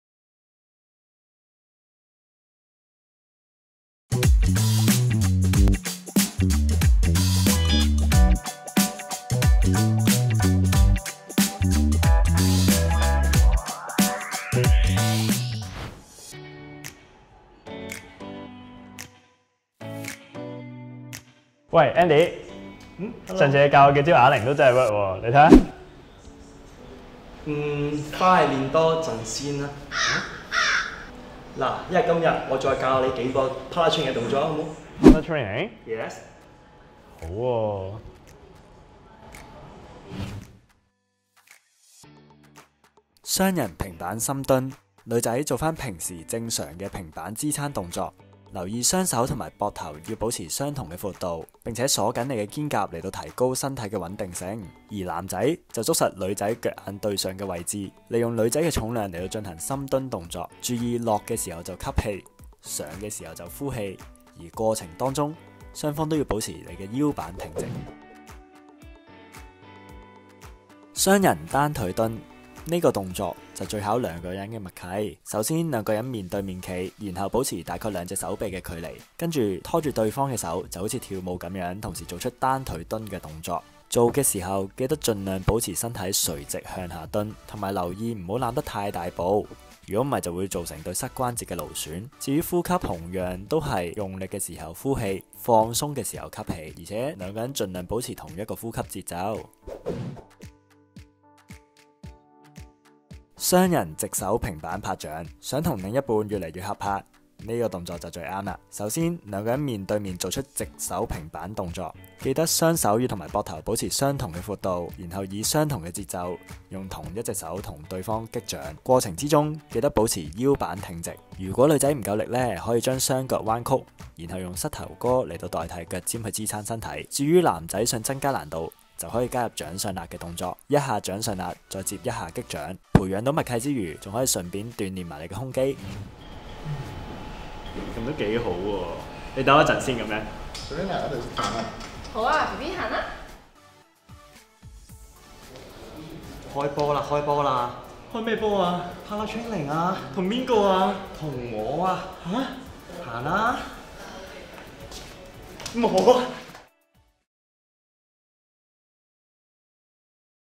Partner Training入門 | 大隻仔健身室 | Esquire HK
一個人健身冇動力？Esquire搵嚟專業健身教練Andy，傳授大家三招Partner Training動作，今個周末搵埋另一半一齊練啦！